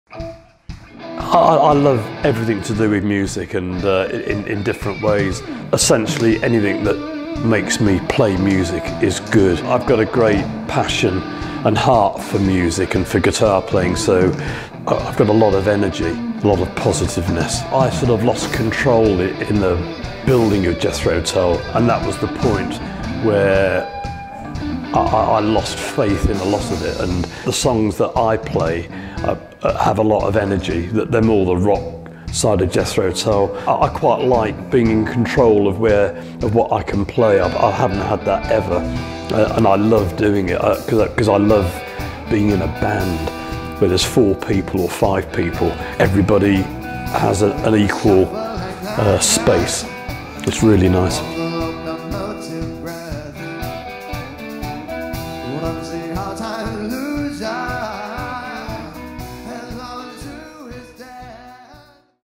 Martin Barre Interview Trailer
A short teaser for the forthcoming Martin Barre interview.